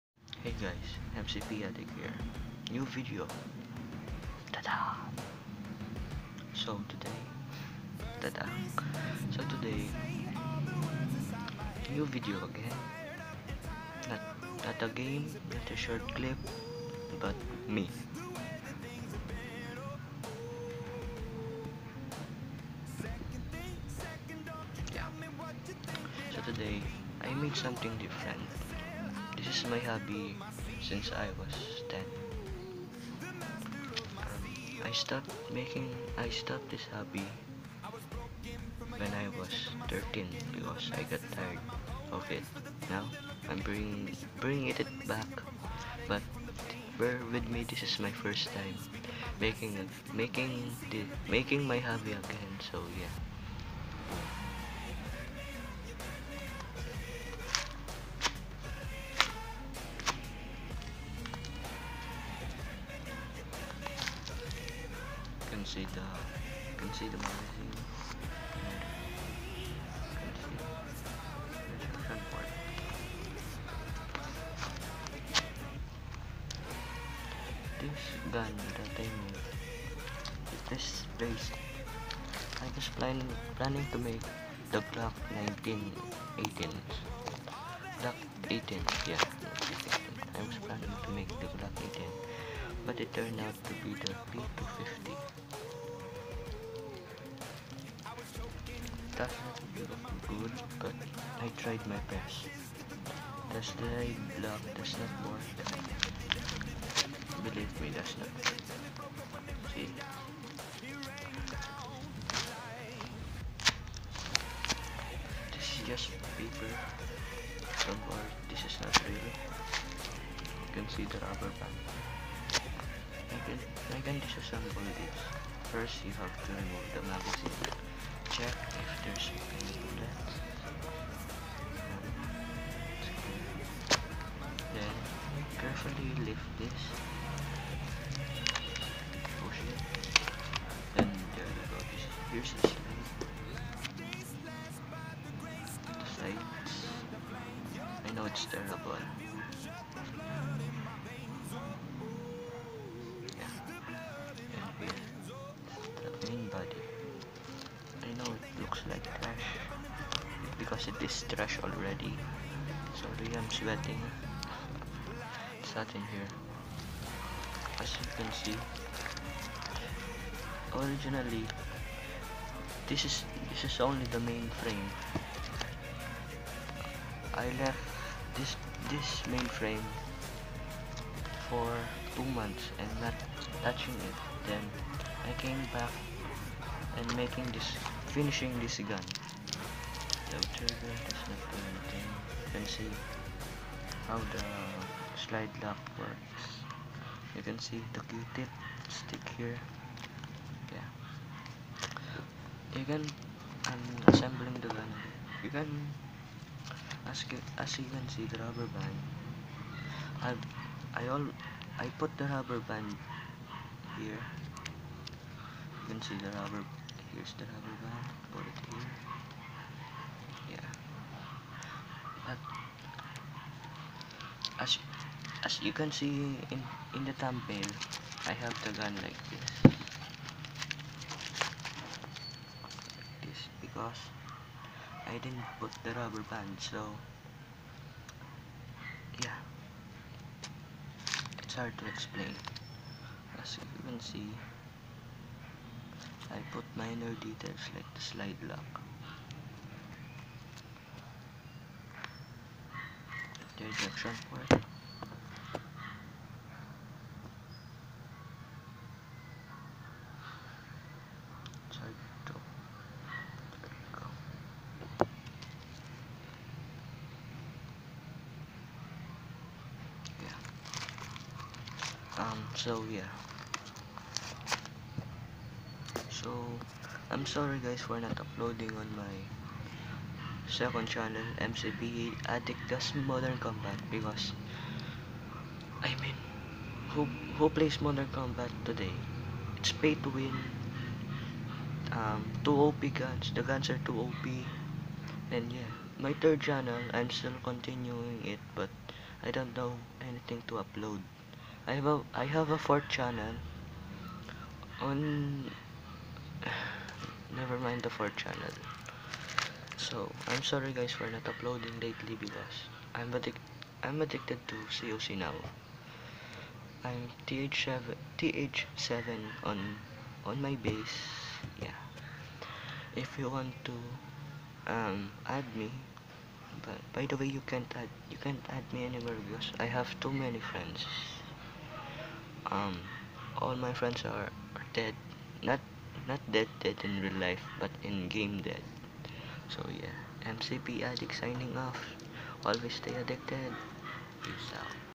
Paper Glock 18/ P250 sorta + Read Desc
Heyyyyyy. it's me! Yah Wooh! Back again! So yah I did something different today and I made a Paper Glock/P250 this was supposed to be done last 2 months ago but I got lazy lol Yah Btw I know it's TRASH but bear with me I will make this better in the future

I'll try to make a tutorial if you guys request

McPeAddict Signing Out

STAY ADDICTED